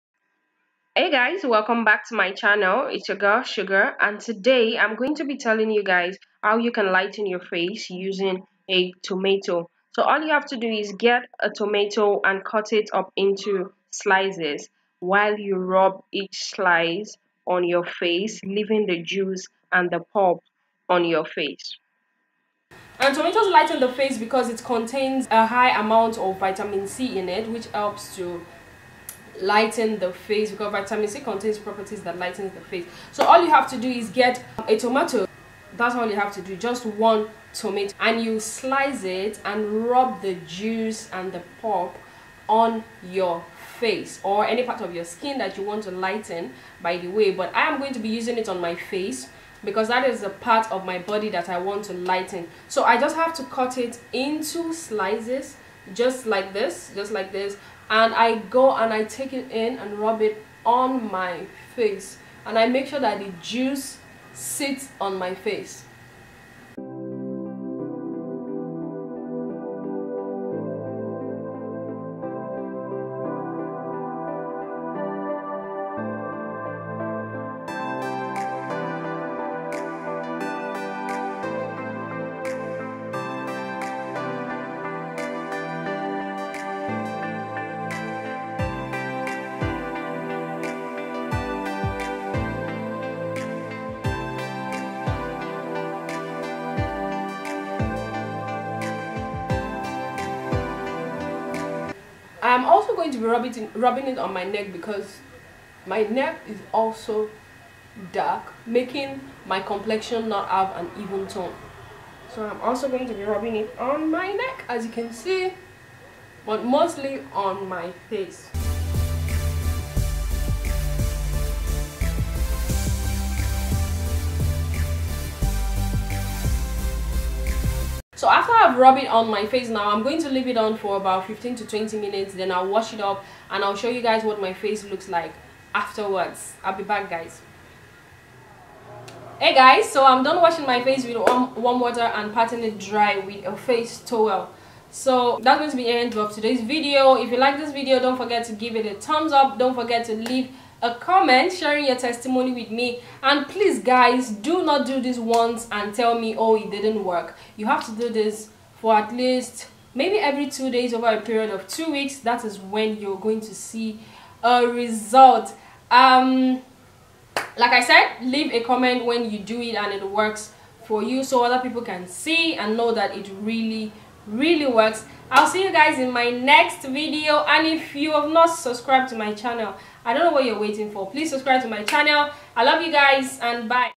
How to lighten the skin using Tomatoes// Natural skin lightening an brightening ingredient.
Tomatoes are one of the naturally active ingredients used to lighten the skin.

It is cheap and can basically be found everywhere.
it is to be rubbed on the part of the skin intended to be lightened,
for twice in a day for a period 2 weeks at least.